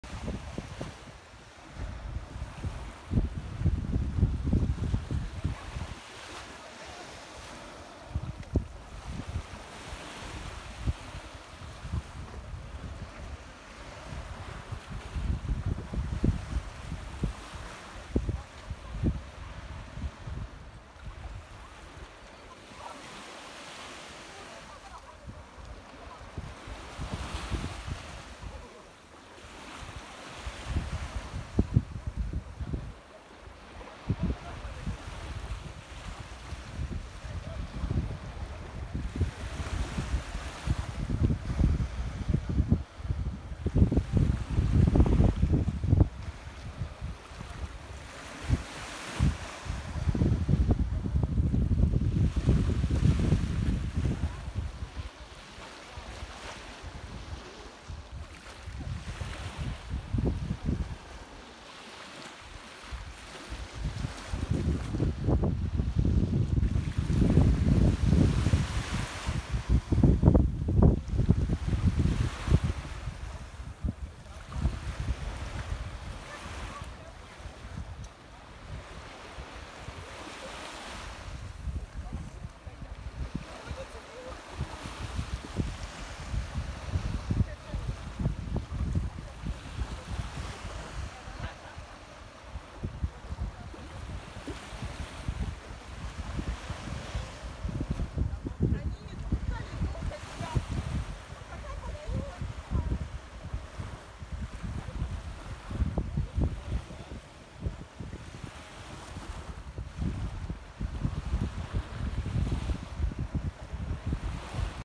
Одесса, море, 04.07.24
Данное видео прилагается к фотографиям и предназначено ( по большей части)  для  участников  Одесского форума.
Как  показательная видеофиксация о состоянии морской воды.